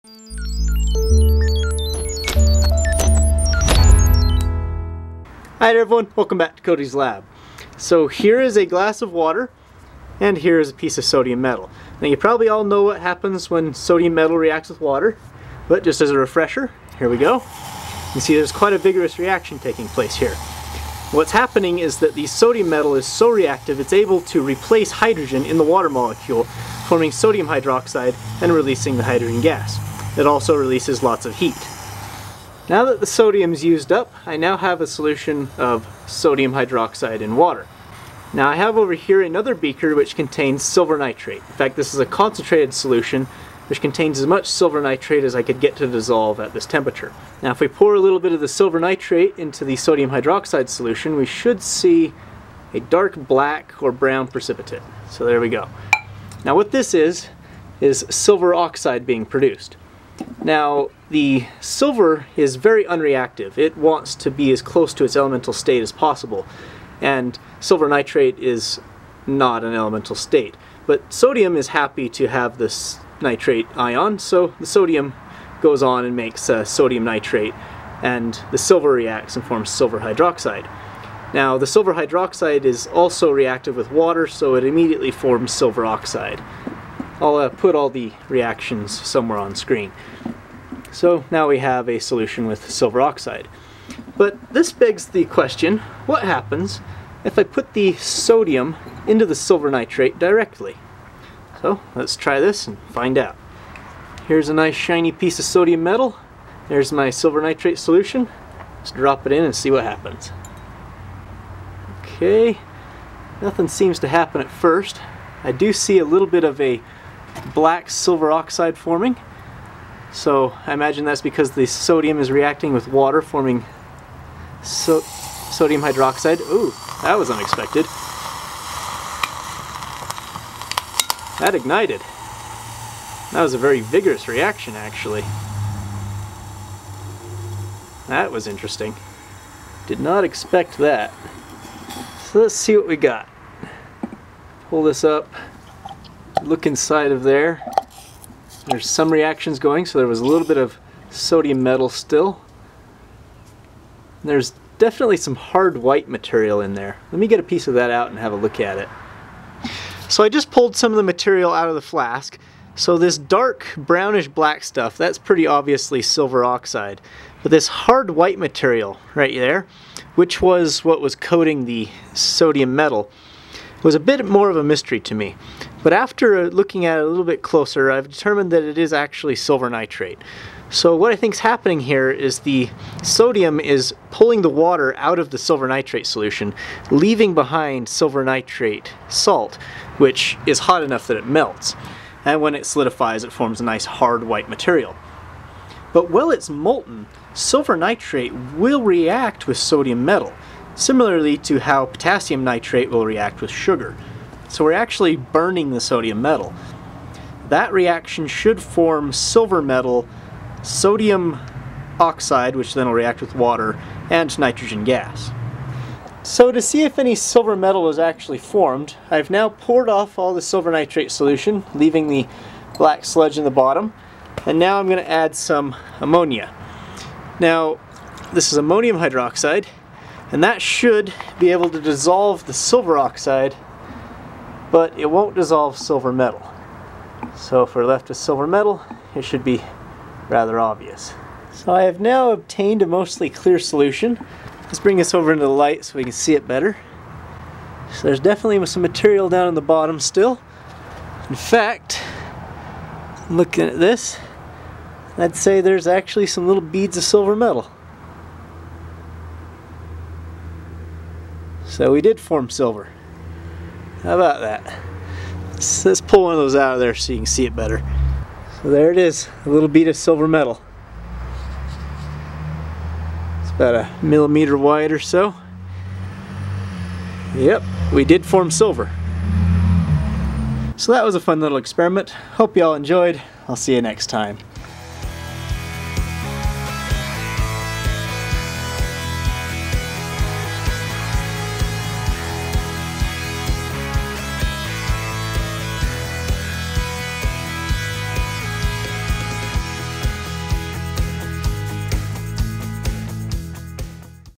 Sodium Metal vs Silver Nitrate
I put some sodium metal into a concentrated solution of silver nitrate.
Some silver metal may have formed from reacting with the silver nitrate directly but it would not have formed solid balls of silver.